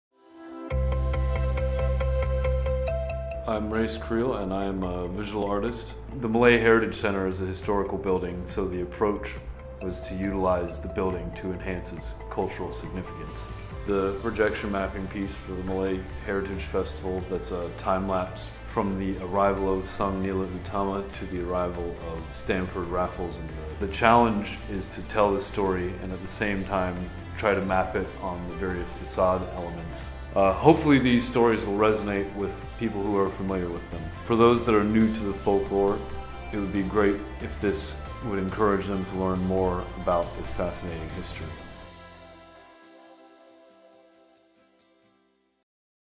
[MCF2019] Balada Tumasik
Learn about the story of Singapore through Balada Tumasik (Ballad of Tumasik).

Watch as the Malay Heritage Centre come alive with an illumination of Singapore's rich history of folk stories and legends! Catch the light show happening daily from 730PM onwards, till 2 November.